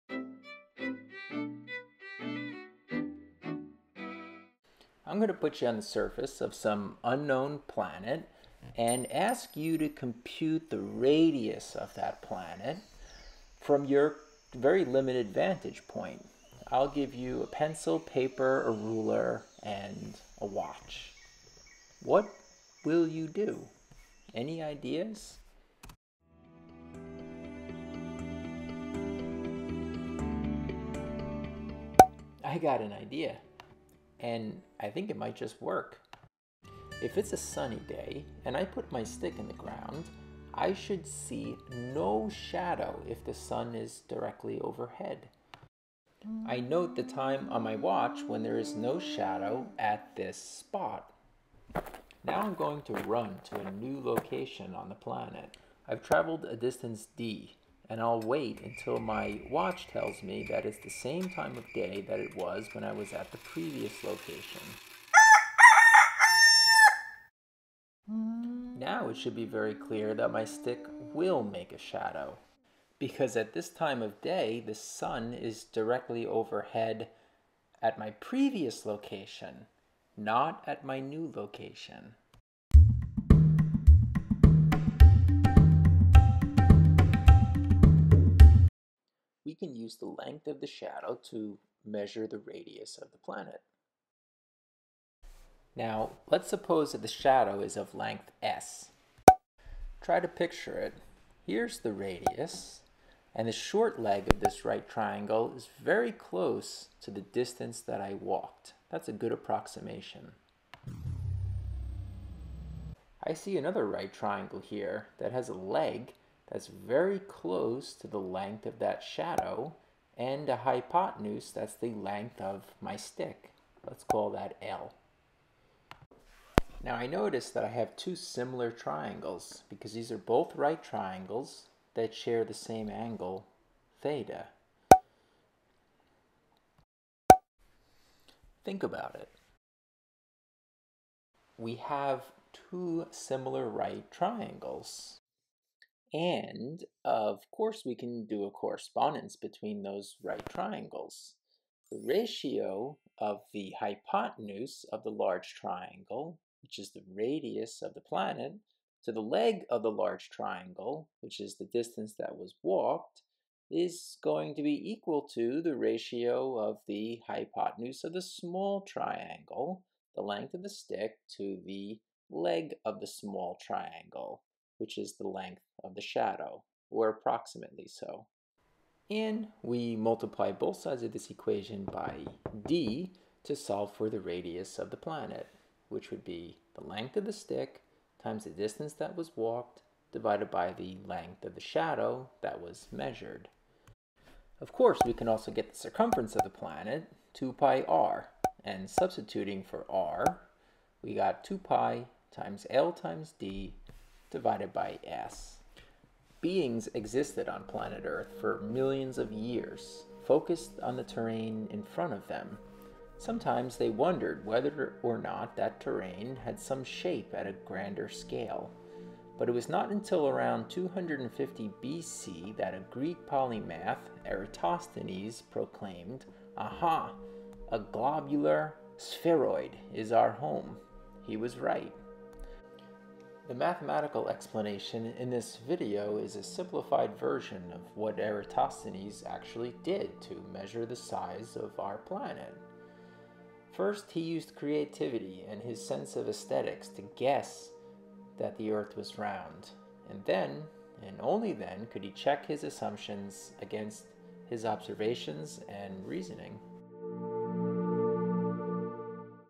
How Eratosthenes discovered the radius of the earth
This video explains how humans managed to measure the circumference of the earth long long ago using simple math, reasoning and assumptions.